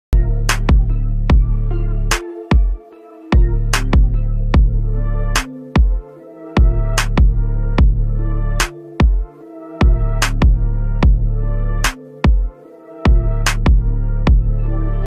2023 Honda BRV 5-Star Safety⭐| Prime Drive
The All-new 2023 Honda BR-V was awarded a 5-Star Safety Rating and is one of the safest MPV for your family.

Source: ASEAN NCAP